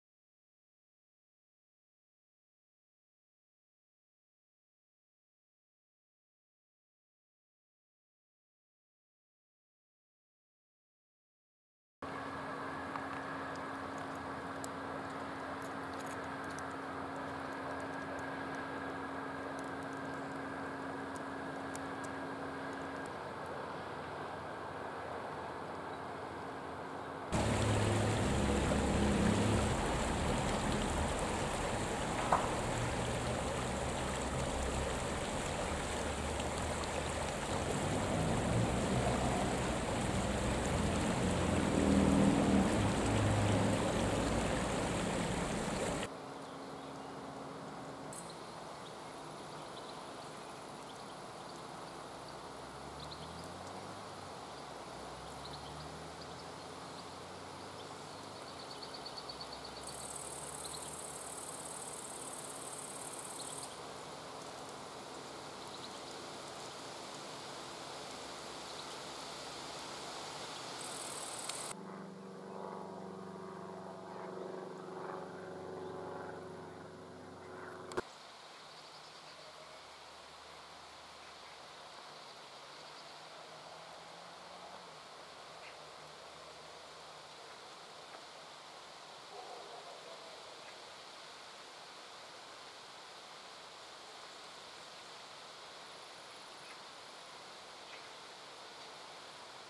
Canards colvert, famille, couple, et groupe important
Canards colverts filmés dans des lieux différents. Une canne et ses petits qui ont déjà grandi en juillet, un couple en plumage postnuptial en septembre, et un groupe très important de canards en août.